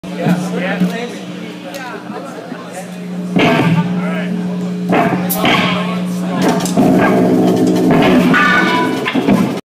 Cecelia Frightwig/Cole Palme: The Re-Creation Of Punk.
Cole Palme of Factrix clasps the hand of Frightwig's Cecelia Kuhn from the stage on 22 September 2013 at the SF Punk Homecoming, Club Verdi, a punk Sistine Chapel for a punk version of  Michaelango's Creation Of Man.